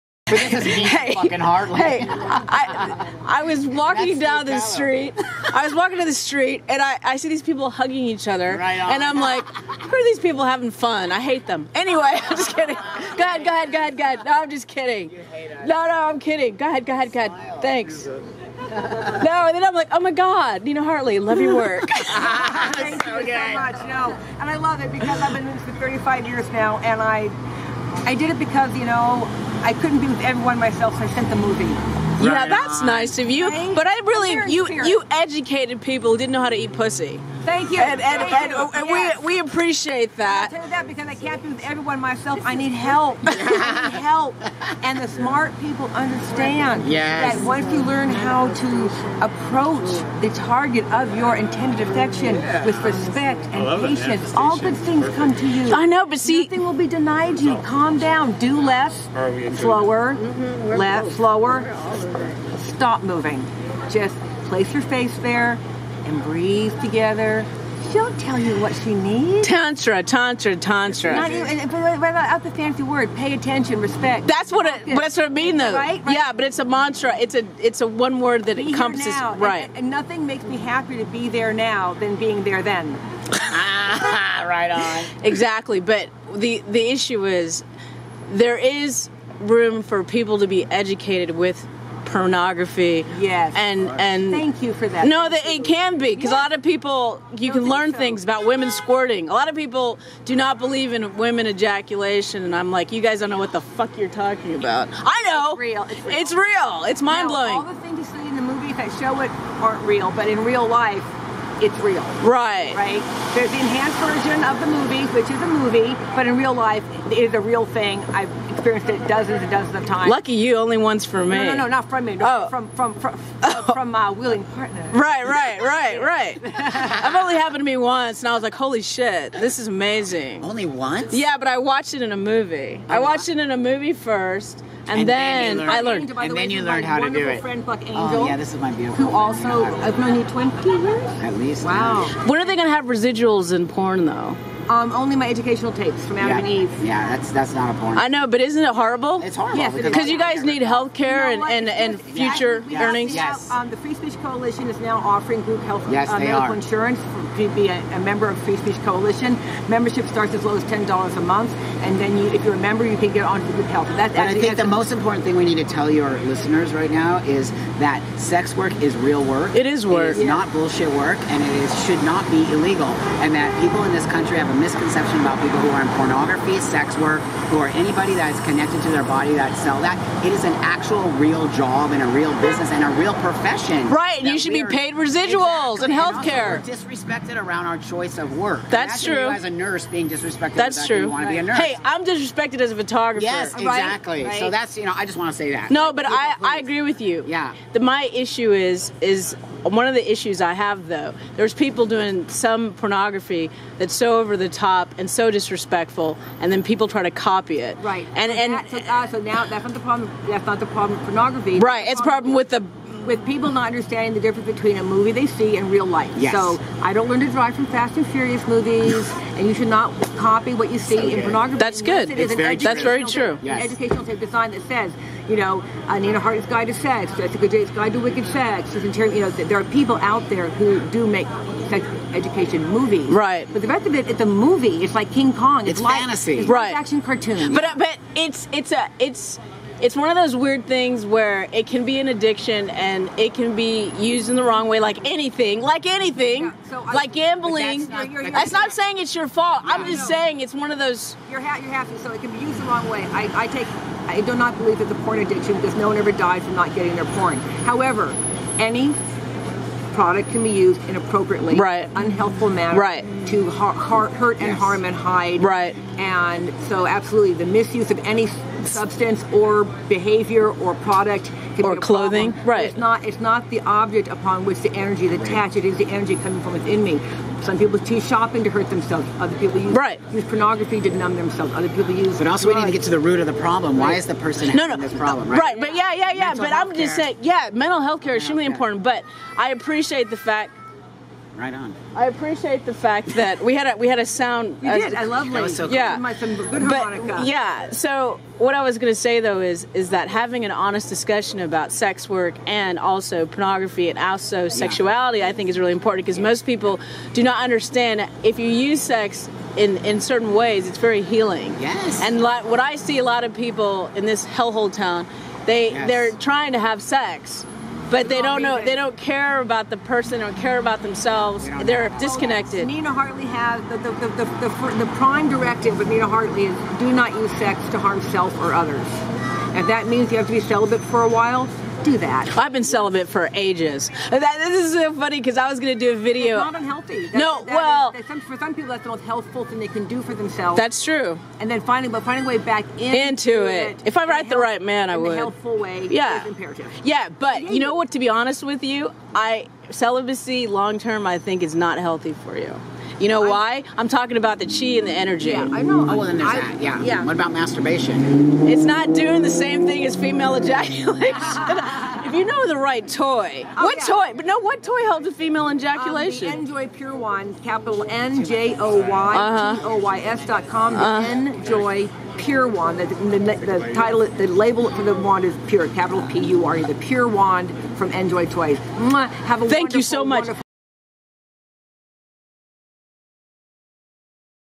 Nina Hartley and Buck Angel Chat With KAT
Two Legends of Porn Nina Hartley and Buck Angel chat with KAT about SEX.